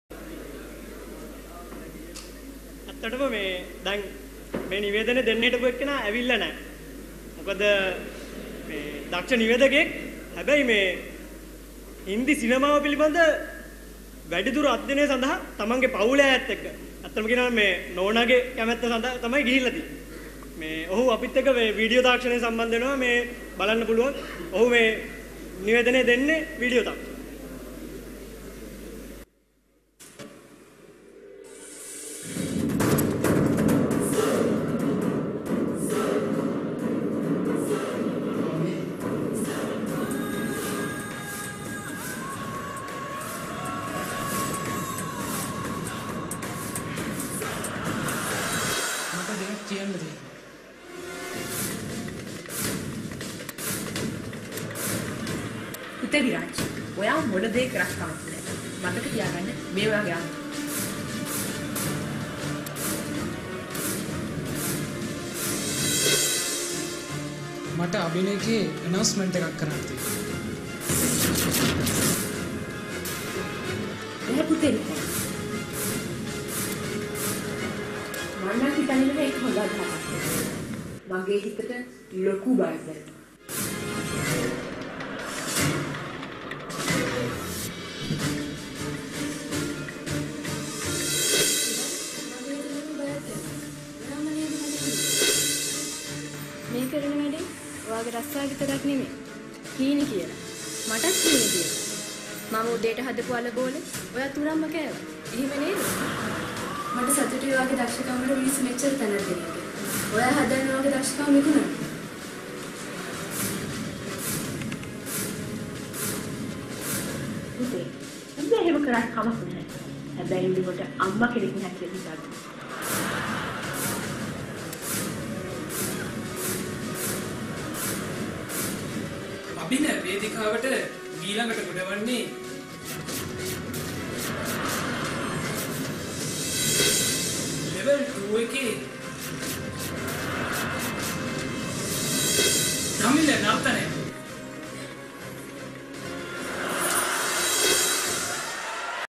Abhina 2017 - Mega Drama
Abhina 2017, the annual concert organized by the Department of Electronic and Telecommunication Engineering University of Moratuwa was held at 29th March 2017 at Civil Auditorium with the honorable participation of Vice Chancellor Prof. Ananda Jayawardhana as the Chief Guest and Mr. Gigiraj Kaushalya as the Guest of Honor.